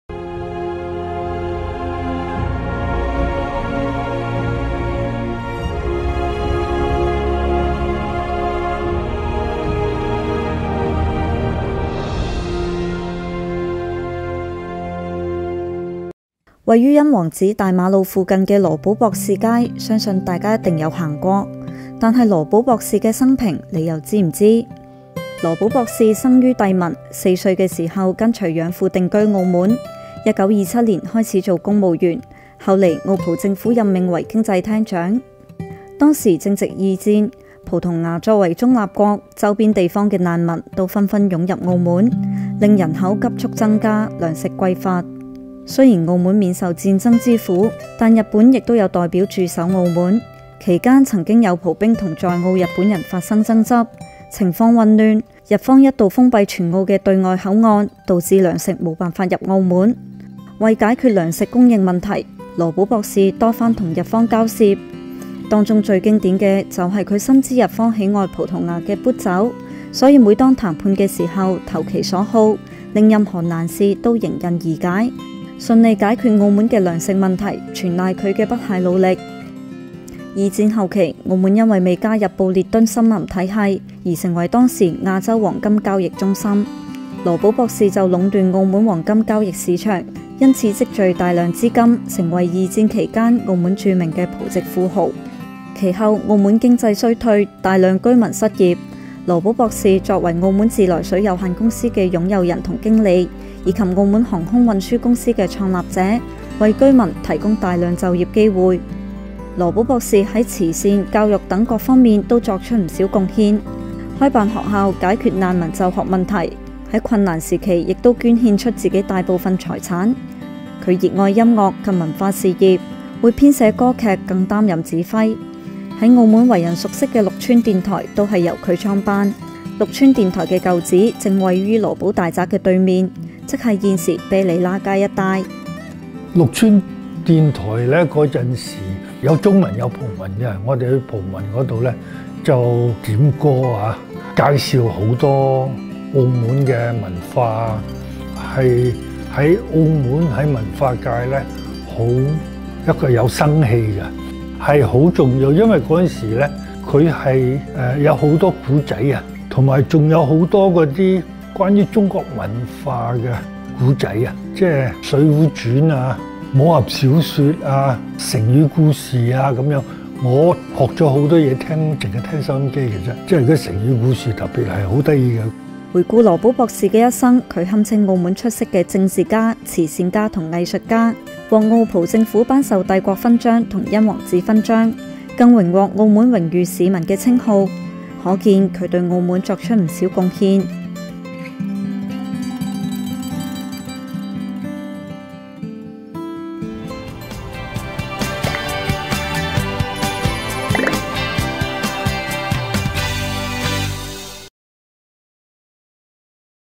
羅保博士街的“羅保”是誰？
【羅保博士是誰？】位於殷皇子大馬路附近的羅保博士街，相信大家一定有行過，但是，羅保博士街中的“羅保”到底是誰？堪稱澳門出色的政治家、慈善家和藝術家的他，大家又知道他的生平嗎？想知“羅保”到底是何方神聖，一於去片了解！